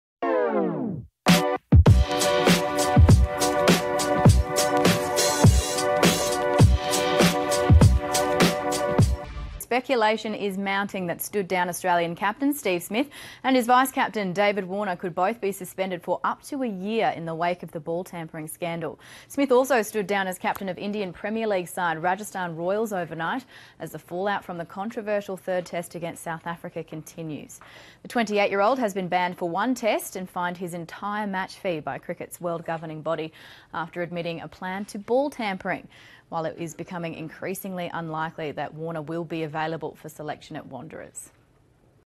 Smith Got Sacked from Australian Team
Smith Got Sacked from Australian Team by CA after Ball Tampering incident